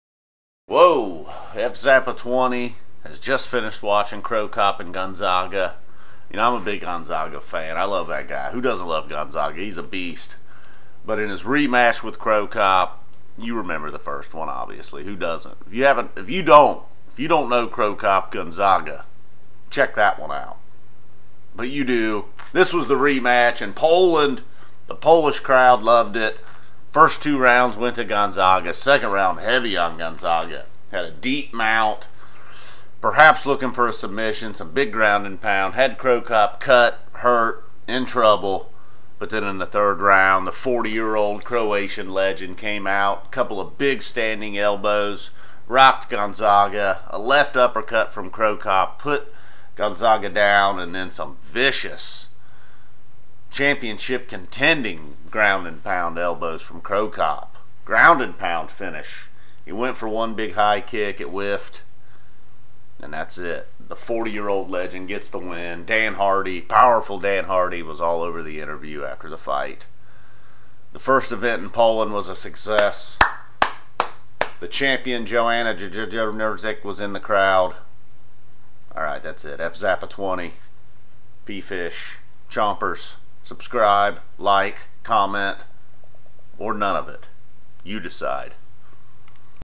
Cro Cop vs Gonzaga 2 - Chomper Reaction
Mirko Cro Cop vs Gabriel Gonzaga 2 from Poland at Fight Night 64. Cro Cop with the elbows. No head kicks this time around. 3rd round finish for the Cro Cop. No need for a 3rd fight. Fzappa20 video.

link to the cro cop fight on the podcast with bravo and schaub

Leon Rocky Edwards had 8 second KO vs Seth Baczynski
Seth Baczynski vs Keon Edwards in Poland